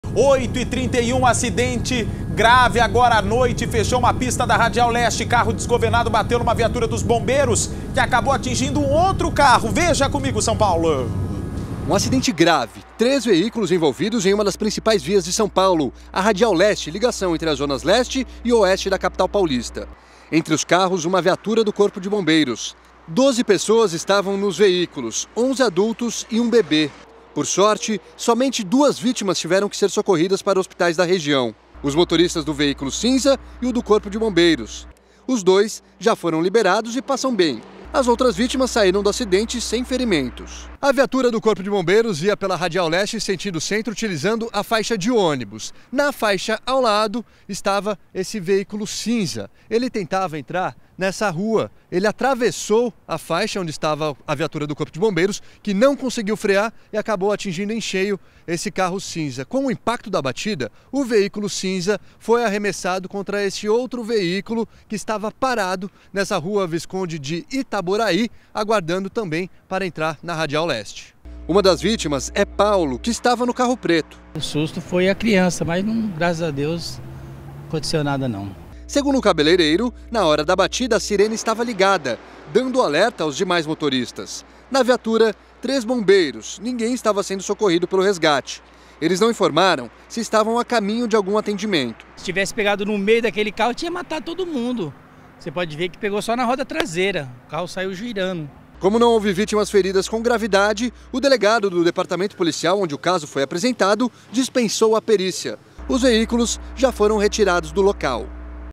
Carro desgovernado bate em viatura do Corpo de Bombeiros na Radial Leste
Um acidente envolvendo dois carros e uma viatura do Corpo de Bombeiros deixou duas pessoas feridas, na Radial Leste, em São Paulo. Um dos veículos atravessou a frente da viatura e provocou a colisão.